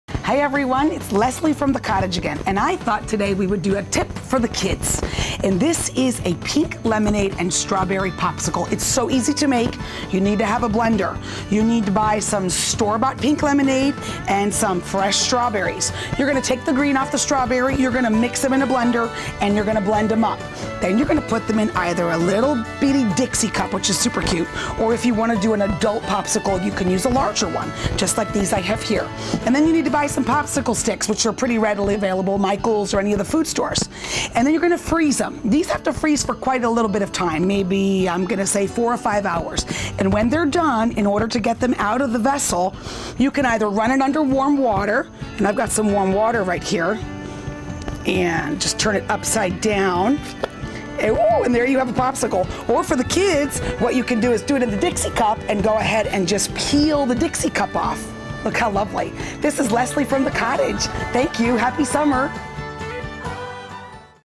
The Cottage Cafe (homemade popsicles)
Chef Leslie Rohland shows us how easy it is to make our own frozen treats!